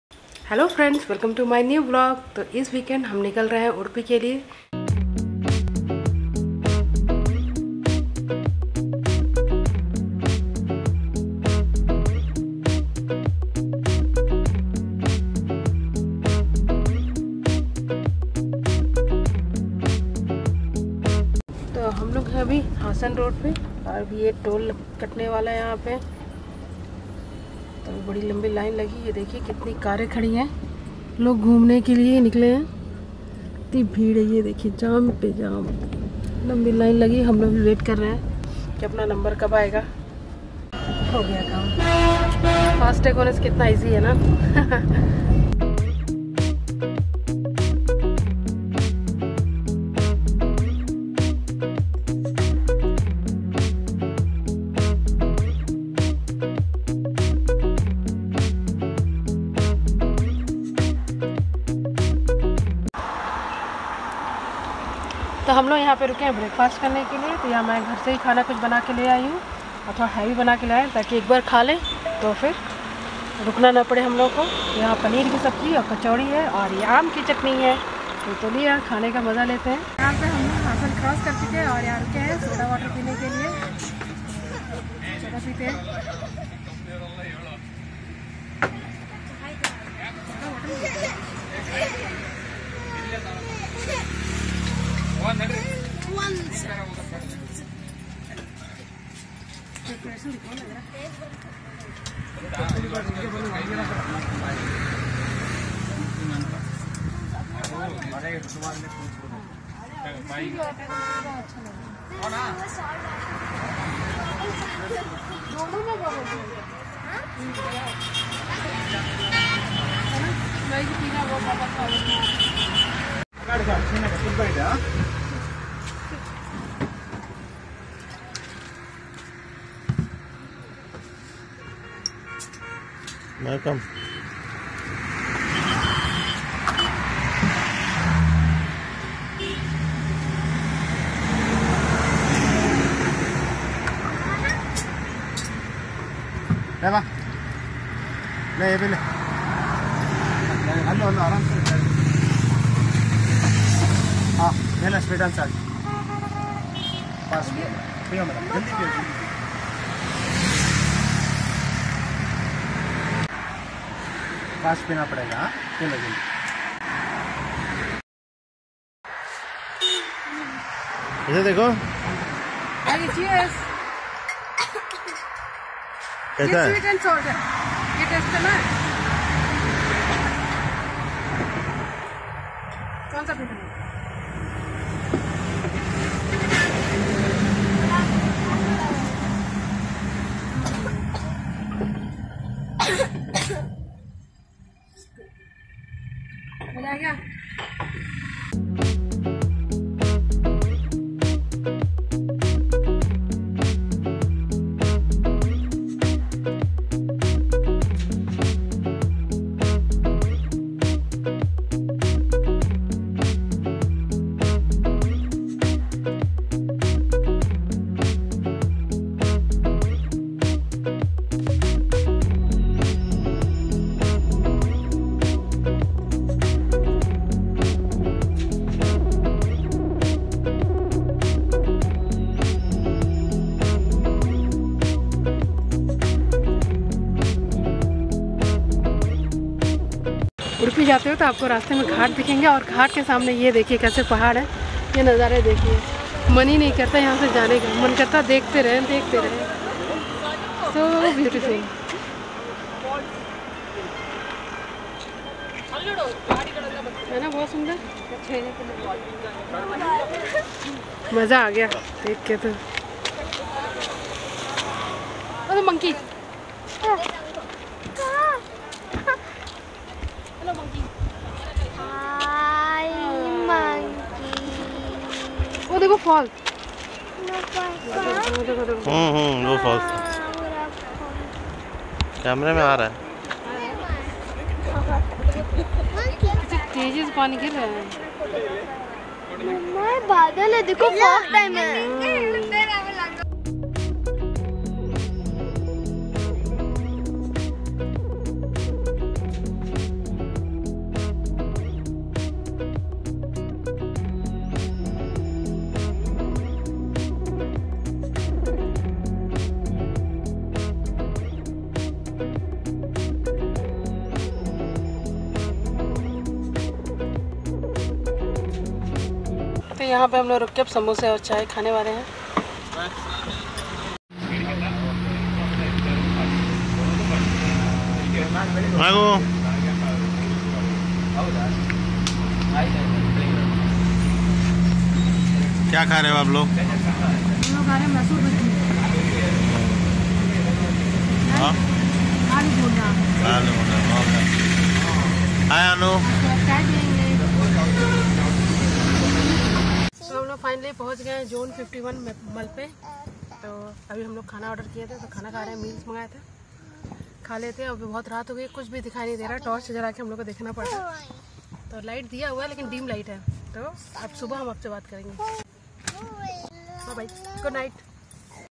Udupi Trip | EP01 : Bangalore to Udupi | Via - Charmadi Ghats | Beautiful scenic drive
Bangalore to Udupi | Via -  Charmadi Ghats | Beautiful scenic drive